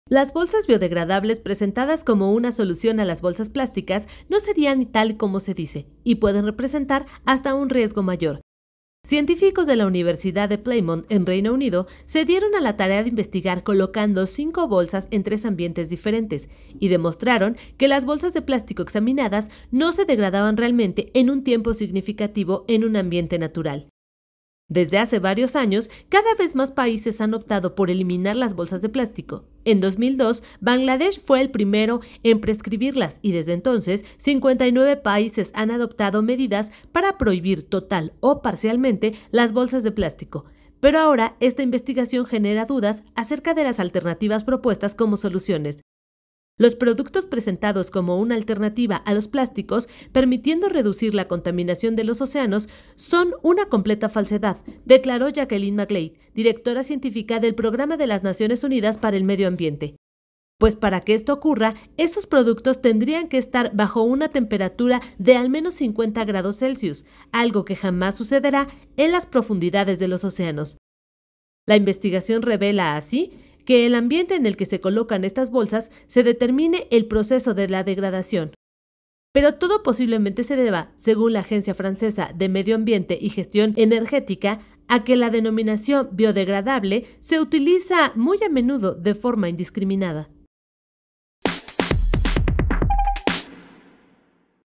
Bolsas biodegradables generan más problemas que soluciones
Un estudio reveló que las bolsas biodegradables, presentadas como una solución a las bolsas plásticas, no serían tal y como se dicen, y puede representar hasta un riesgo mayor.